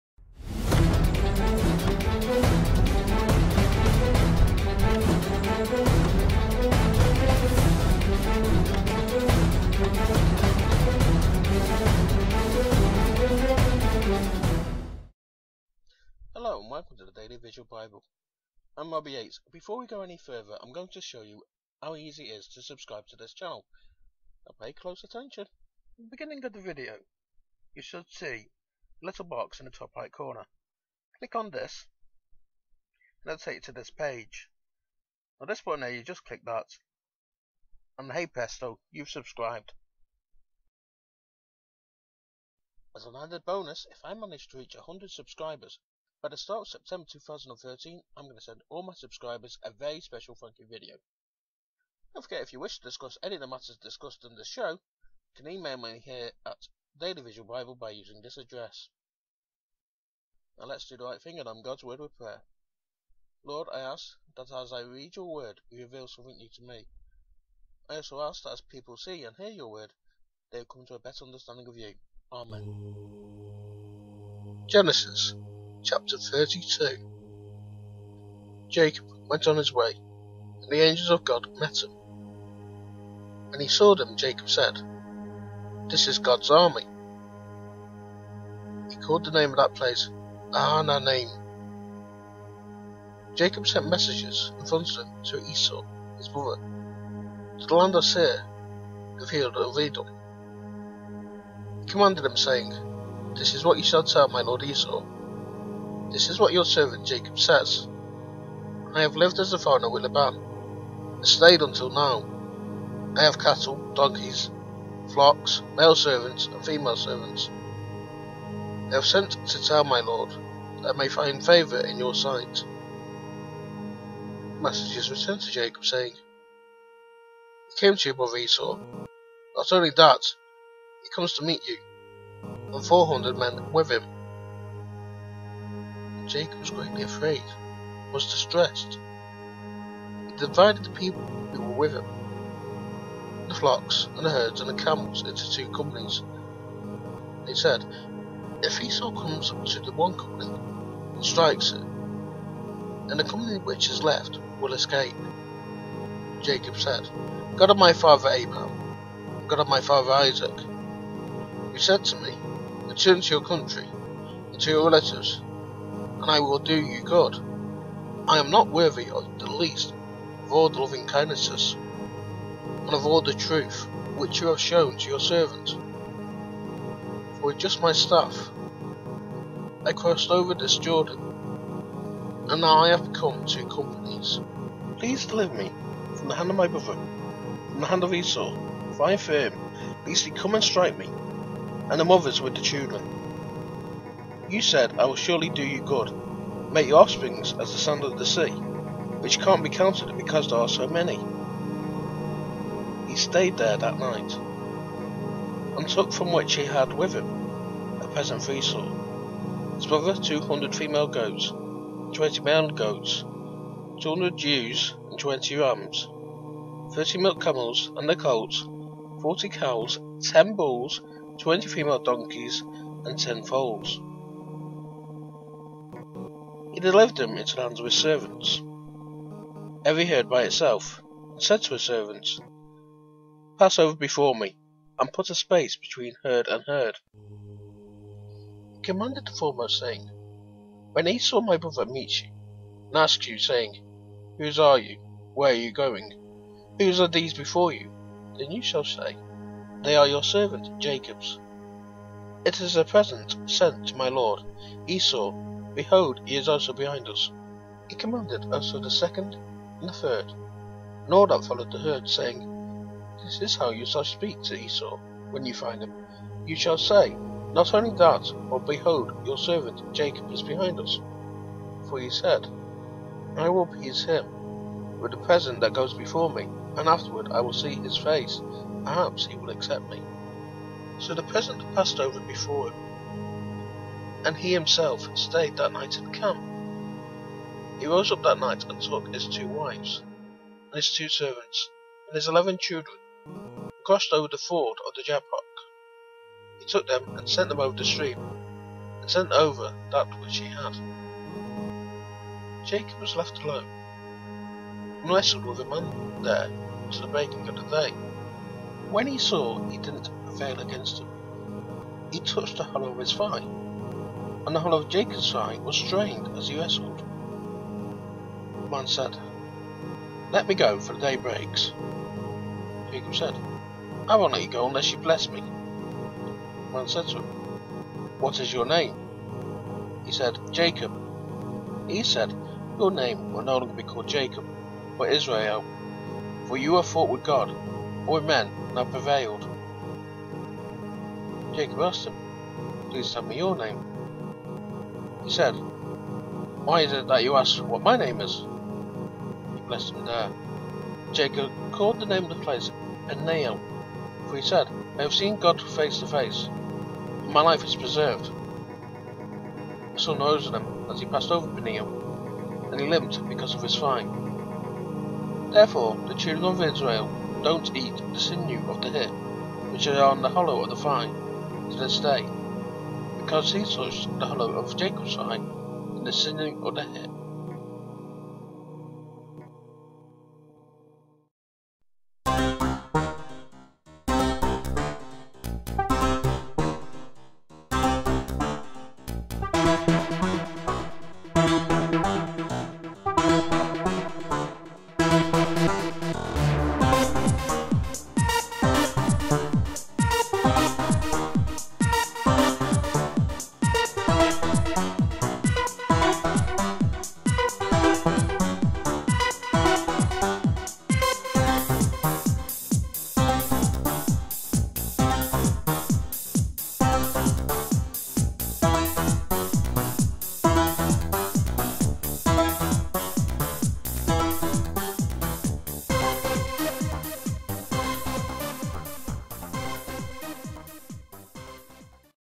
Daily Visual Bible: Genesis 32: Wanna Wrestle?
Jacob prepares to meet with Esau, and then wrestles